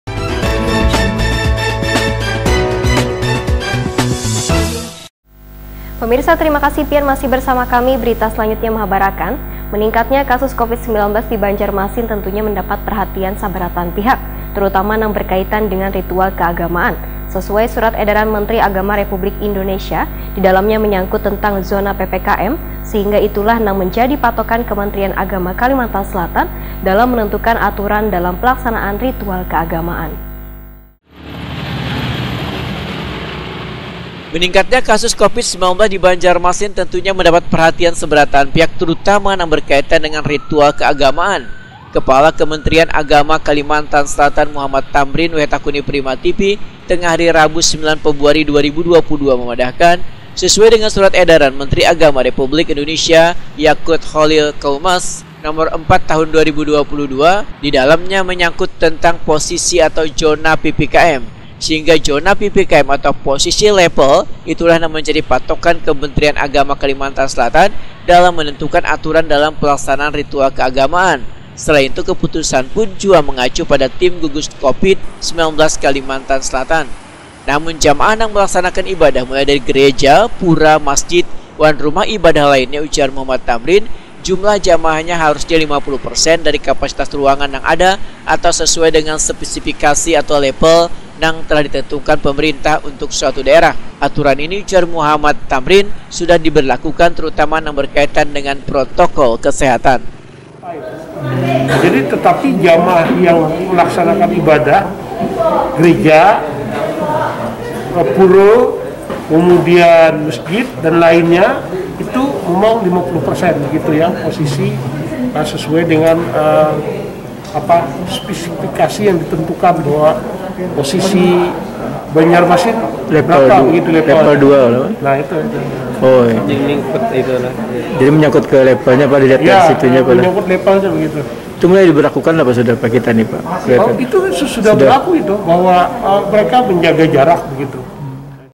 MENINGKATNYA KASUS COVID 19 MENDAPAT PERHATIAN TERUTAMA NANG BERKAITAN DENGAN RITUAL KEAGAMAAN
Meningkatnya kasus covid 19  di Banjarmasin tentunya mendapat perhatian sabarataan pihak//  Terutama nang berkaitan dengan ritual keagamaan// Kepala Kementrian Agama Kalimantan Selatan – Muhammad Thamrin wayah ditakuni prima tv/ tengah hari Rabu 9 Februari 2022 mamadahakan/ sesuai dengan surat edaran Menteri Agama Republik Indonesia - Yaqut Cholil Qoumas.Nomo 4 Tahun 2022 didalamnya menyangkut tentang posisi atau Zona PPKM//